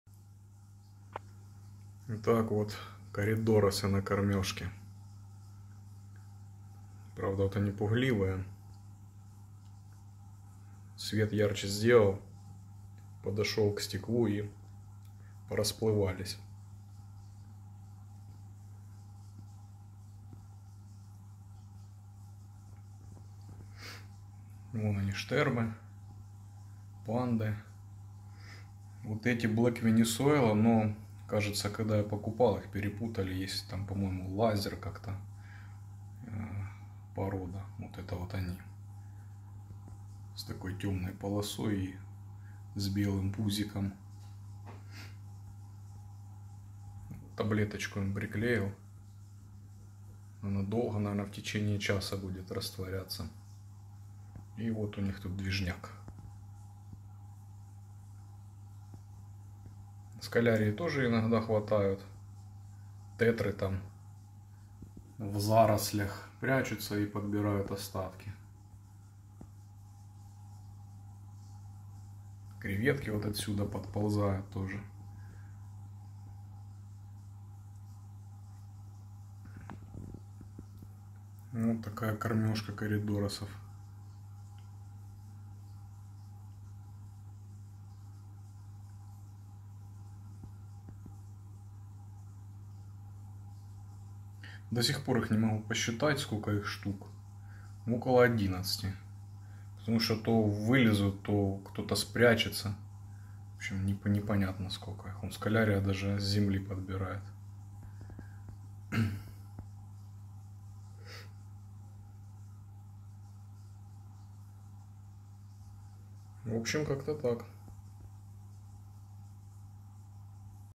Кормежка Коридорасов. Corydoras feeding.
Вот решил показать свою толпу Кориков на кормежке)))